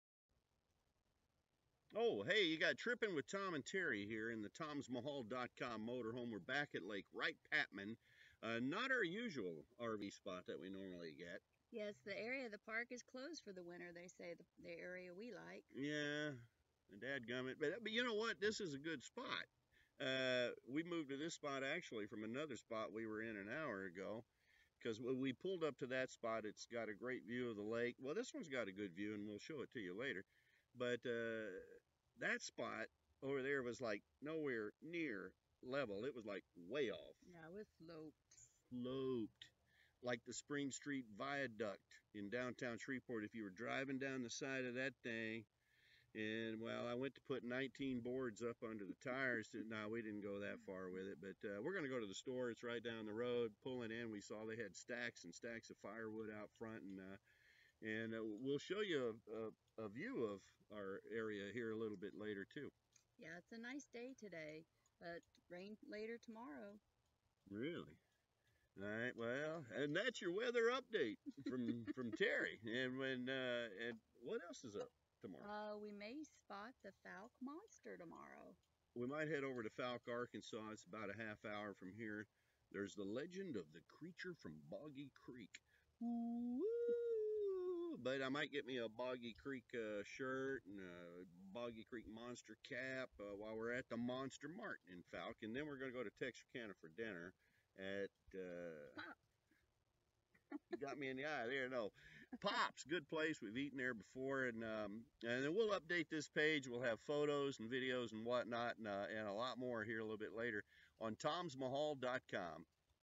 Just Arriving @ Wright Patman Lake
After our arrival at Wright Patman Lake (Texas), we sat down to try our hand at an informative video. It includes Teri's weather forecast, Pops Place (Texarkana), The Creature From Boggy Creek (Fouke, Ar) & more! Other area info & photos on our Wright Patman page in the Journal @ TomsMaHauL.com